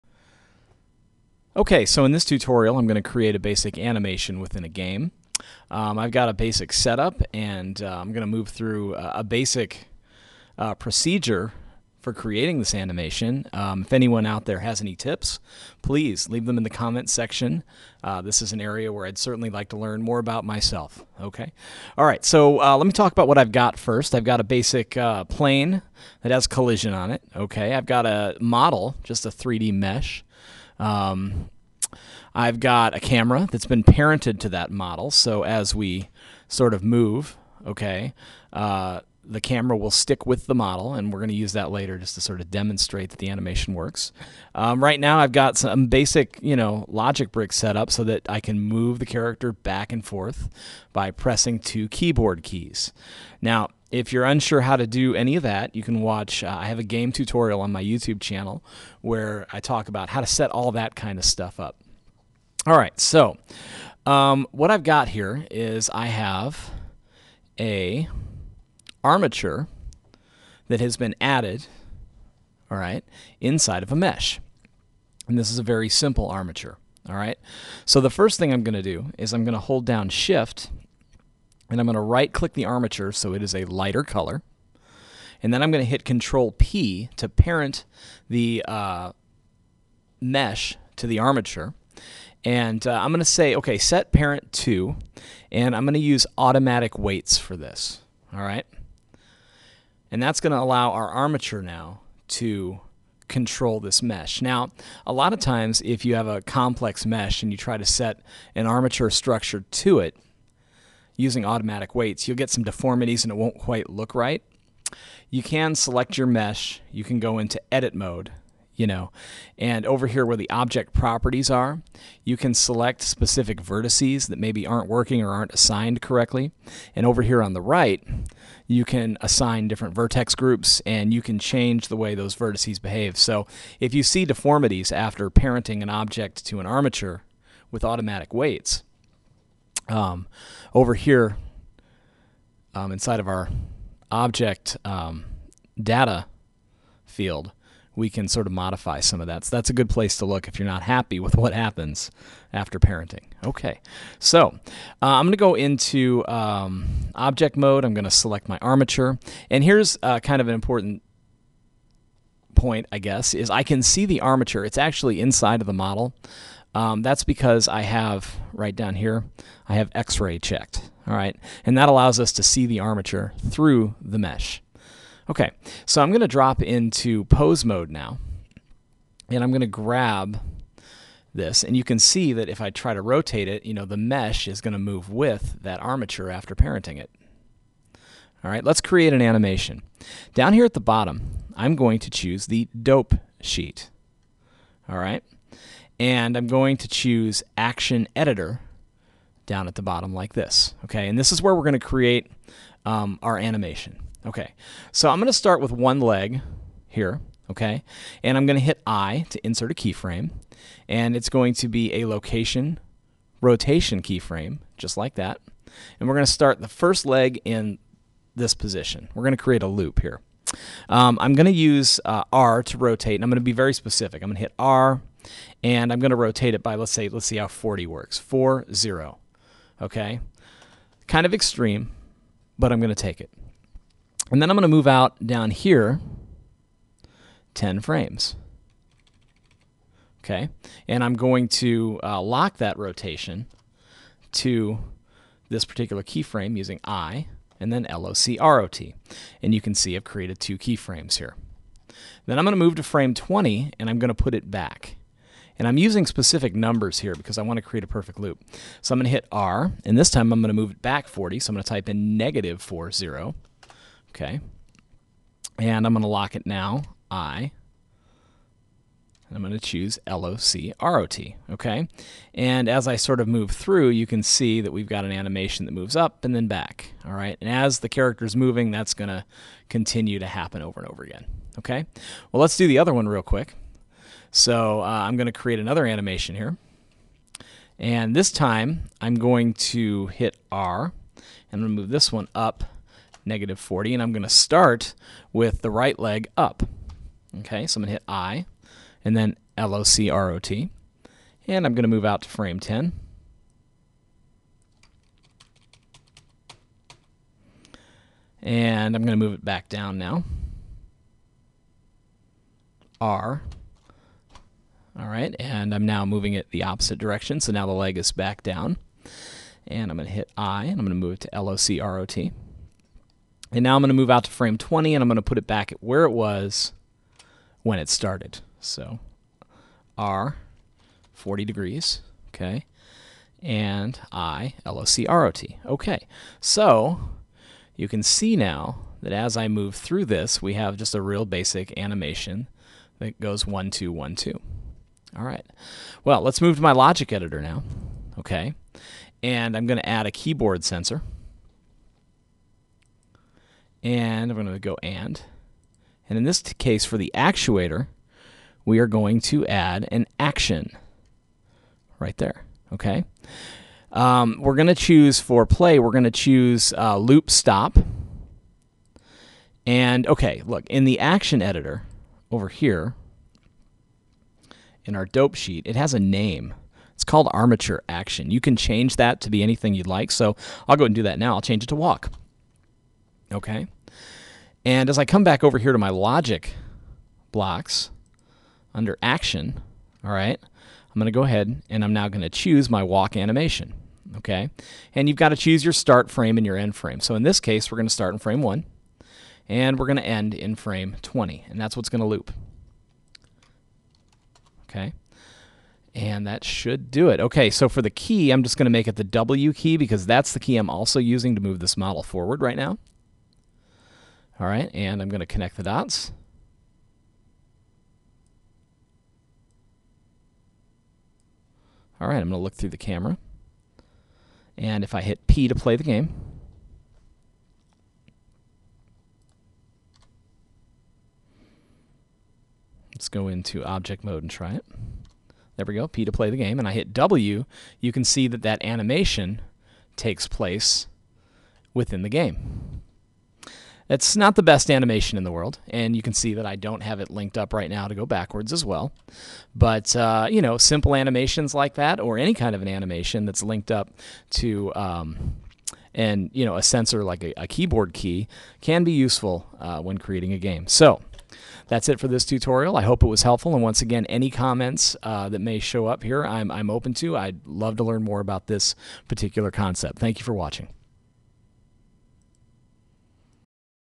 Blender: In Game Character Animation
In this video I create a simple animation that can be used within the Blender game engine.